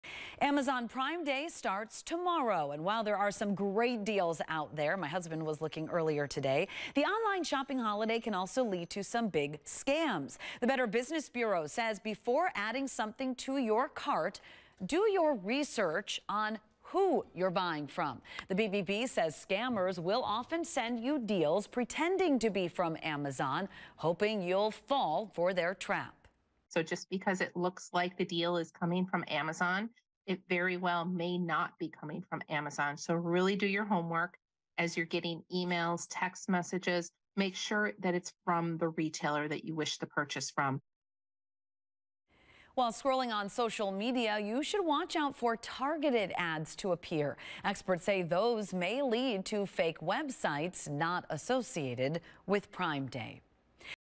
BBB offers consumers tips to avoid online shopping scams ahead of Prime Day
Amazon Prime Day starts tomorrow, and while there are some great deals out there, the online shopping holiday can also lead to some big scams.

The Better Business Bureau (BBB) says before adding something to your cart, do your research on who you’re buying from. The BBB says scammers will often send you deals pretending to be from Amazon, hoping you’ll fall for the trap.

“Do your homework as you’re getting emails and text messages, make sure that it’s from the retailer that you wish to purchase from, not from a scammer,” BBB Regional Director Tiffany Bernhardt Schultz said.

While scrolling on social media, you should watch out for targeted ads to appear. Experts say these may lead to fake websites not associated with Prime Day.